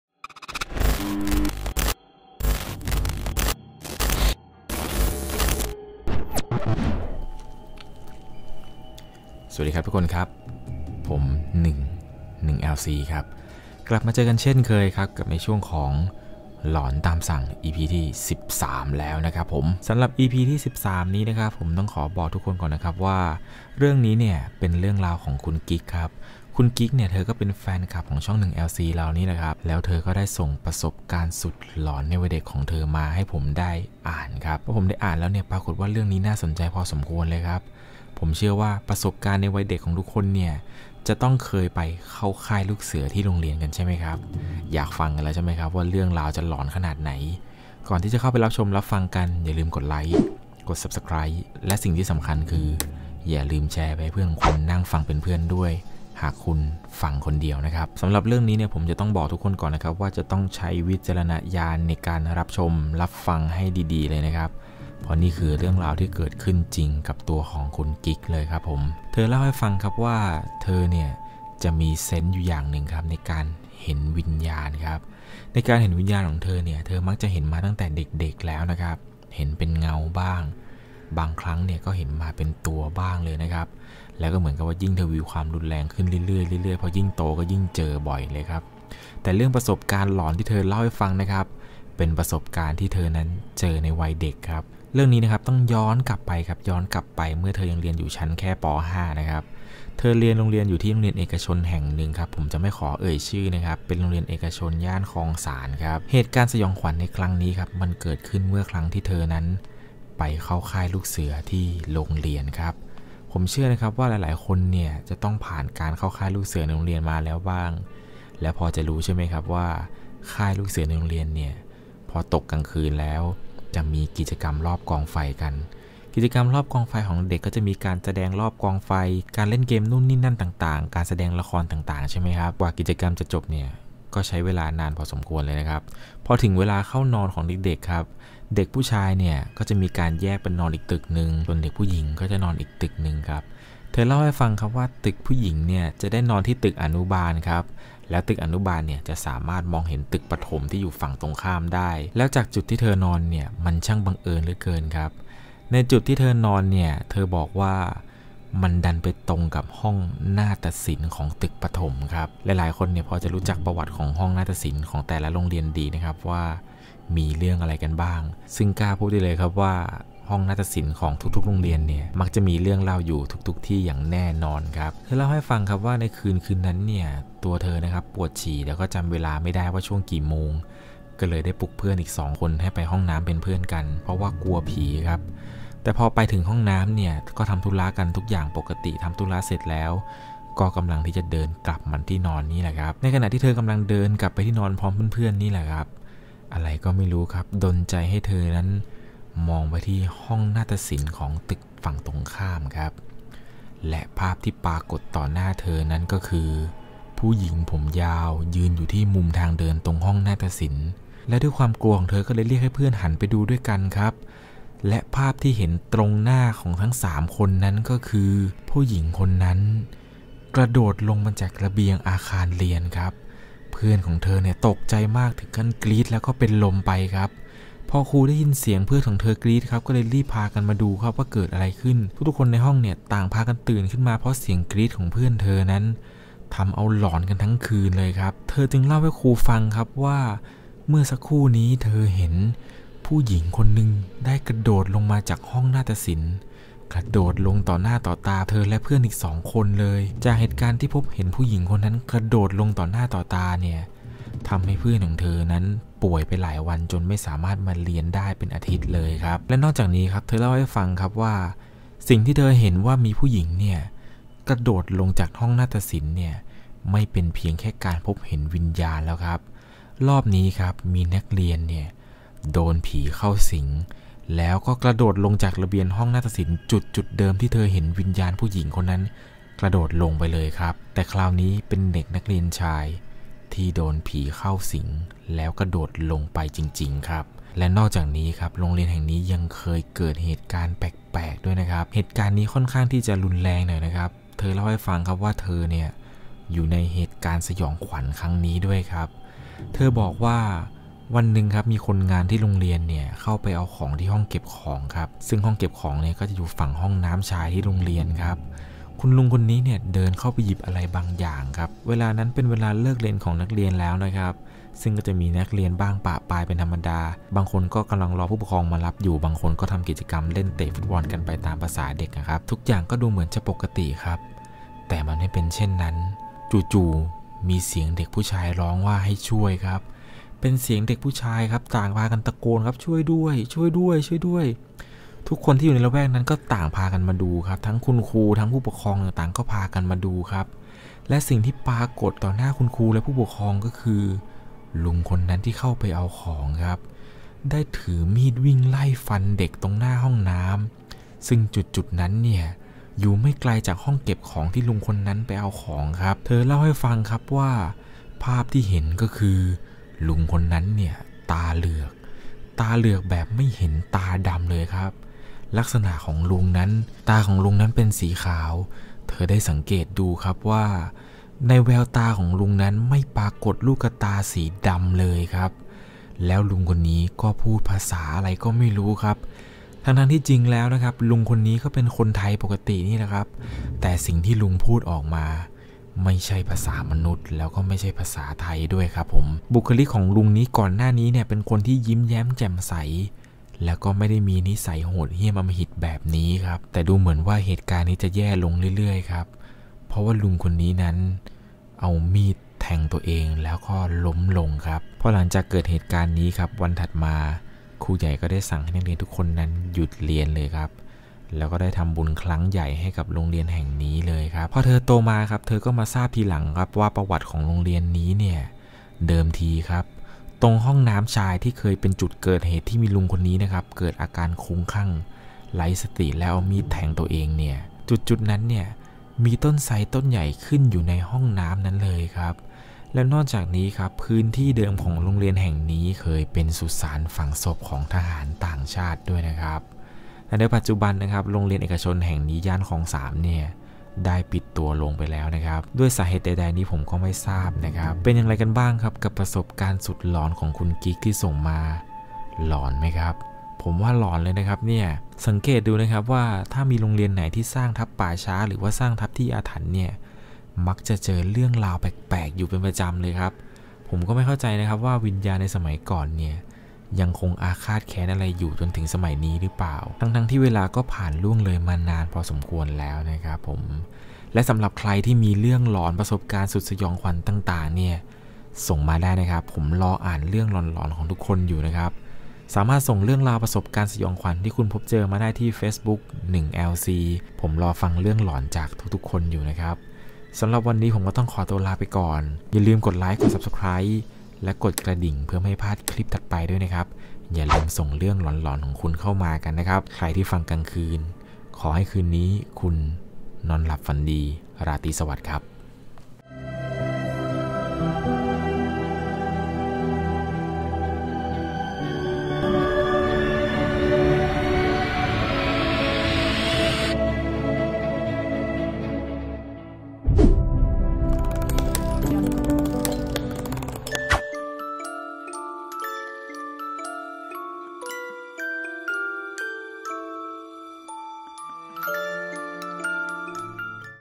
"เจอผีตั้งแต่เด็ก" ในวันเข้าค่ายลูกเสือที่โรงเรียน | หลอนตามสั่ง EP.13 | nuenglc
เรื่องนี้ประสบการณ์สุดหลอนของแฟนคลับที่ส่งมา
จะหลอนจะเฮี้ยนขนาดไหนเชิญรับชมรับฟังครับ
ปล.หากเสียงดนตรีประกอบดังเกินต้องขออภัยด้วยนะครับ 😭
*ใครไม่กล้าดูแชร์เก็บไว้ดูกับเพื่อนก่อนนะ 😘
➤ Youtube chanel : nuenglc
Credit music background :
© Licence ▼